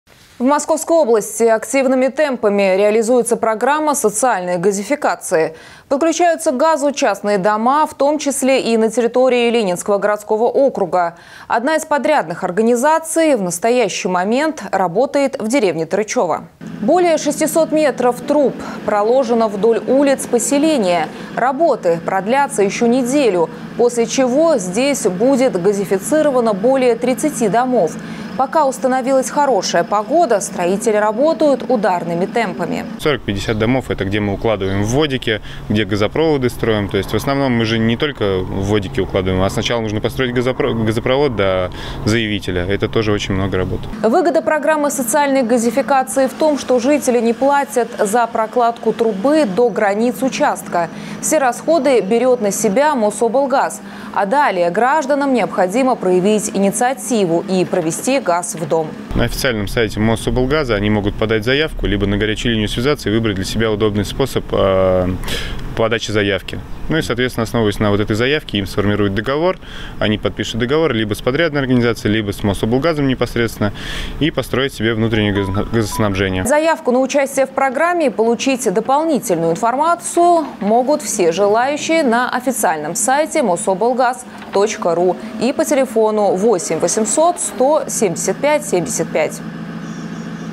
СОЦИАЛЬНАЯ ГАЗИФИКАЦИЯ
В Московской области активными темпами реализуется программа социальной газификации. Подключаются к газу частные дома в том числе и на территории Ленинского городского округа. Одна из подрядных организаций в настоящий момент работает в деревне Тарычёво.